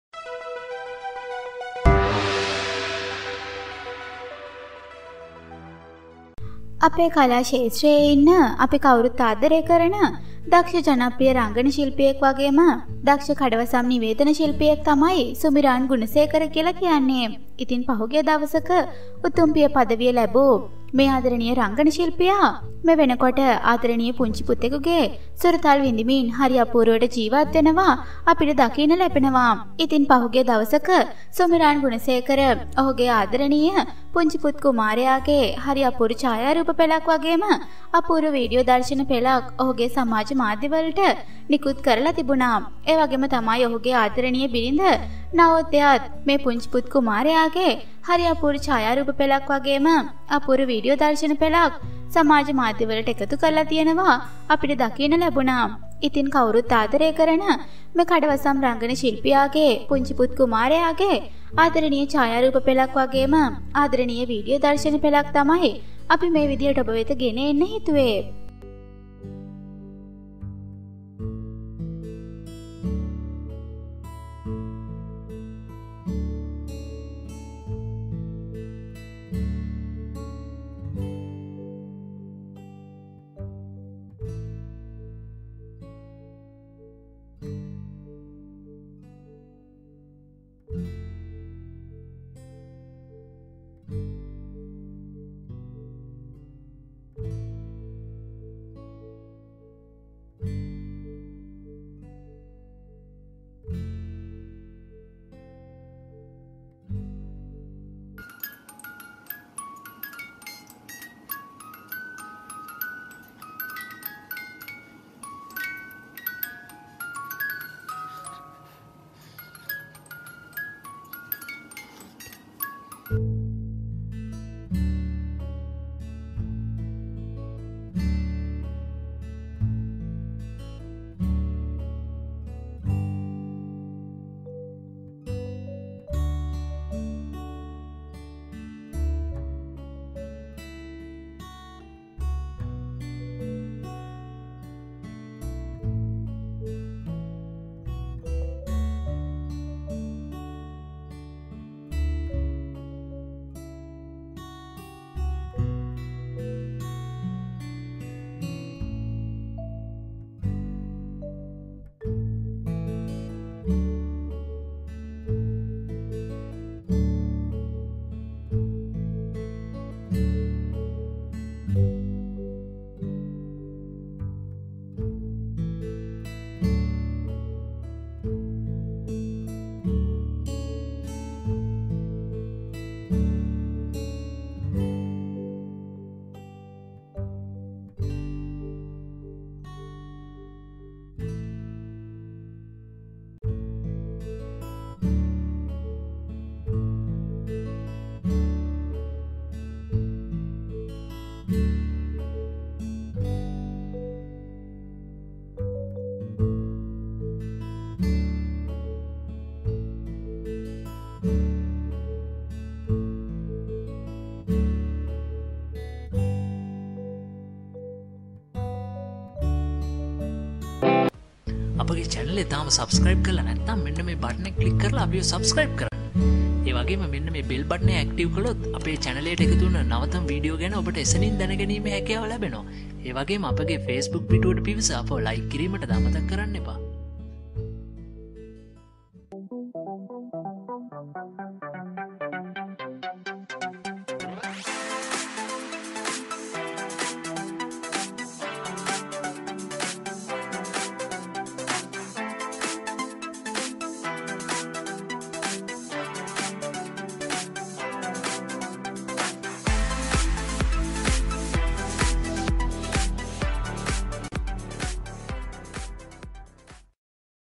සුමිරාන් ගුණසේකරගේ පුංචි පුතු පැටියගේ කවුරුත් නොදැක්ක අපූරු හුරතල් මෙන්න.Sumiran Gunasekara
හා හා පුරා කියලා පිය පදවිය ලැබූ සුමිරාන් ගුණසේකරගේ පුංචි පුතු පැටියගේ කවුරුත් නොදැක්ක අපූරු හුරතල් මෙන්න..Sumiran Gunasekara Baby